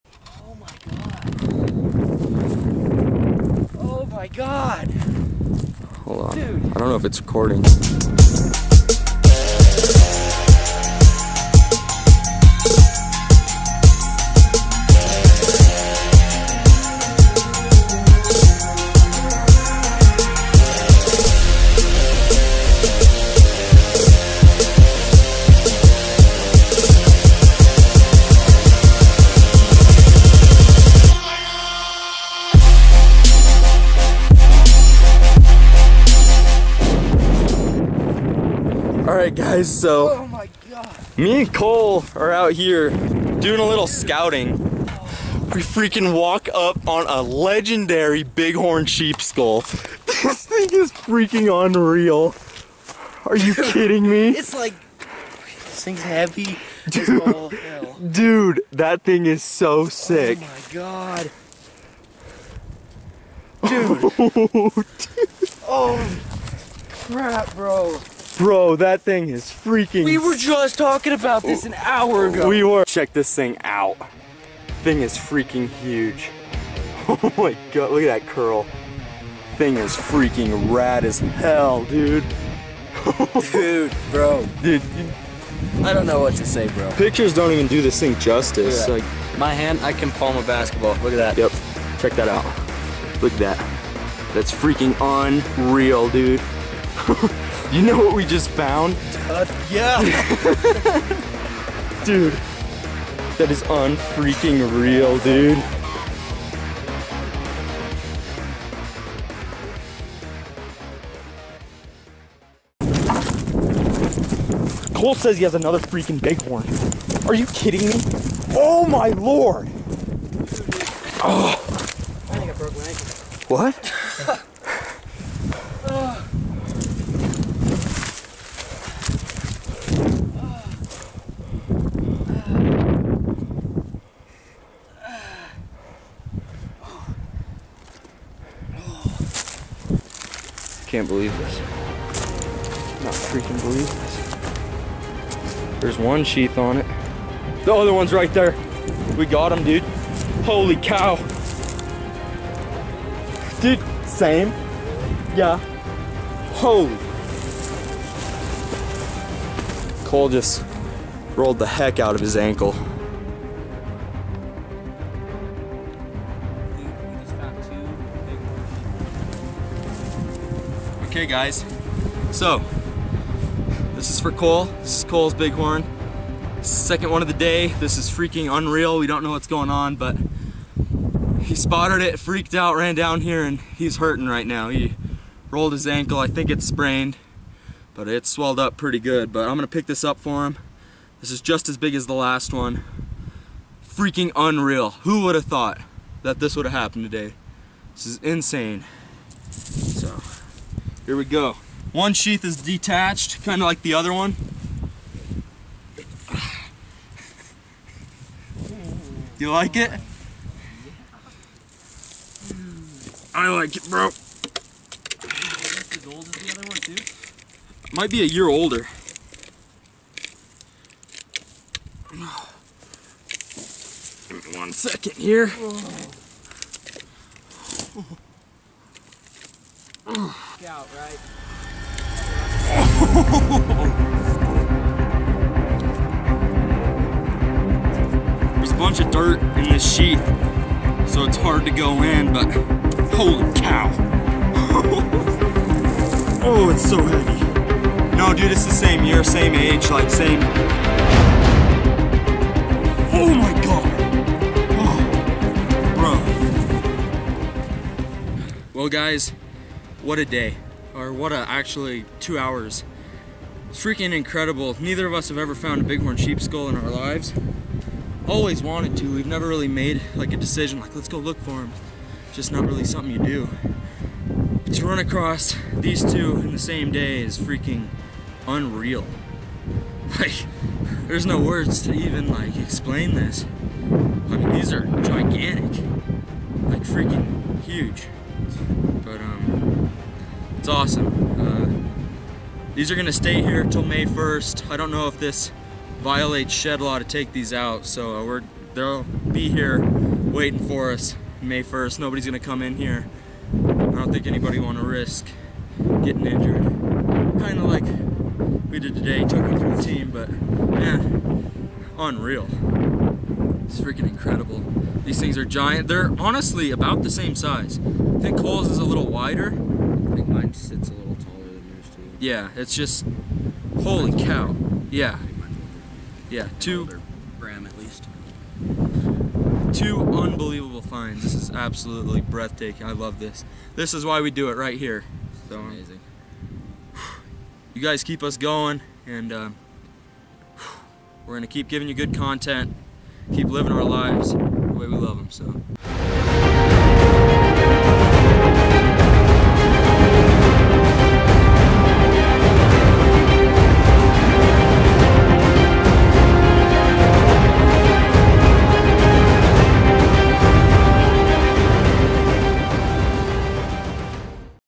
FINDING 2 GIANT BIG HORN SHEEP DEADHEADS IN ONE HOUR
OUT SCOUTING HIGH ELEVATION TERRAIN TOO SEE IF IT WOULD BE WORTHY ENOUGH TOO GET A BEAR TAG FOR , WHEN WE RUN ACROSS NOT ONE BUT TWO GIANT BIGHORN SHEEP DEADHEADS!

AN ACTION PACKED ROLLER COASTER OF COUPLE OF HOURS 👊🏼

A LEGENDARY DAY TOO SAY THE LEAST💯

@MILEHIGHPURSUIT ON INSTAGRAM

@THEUNTAMEDWEST ON INSTAGRAM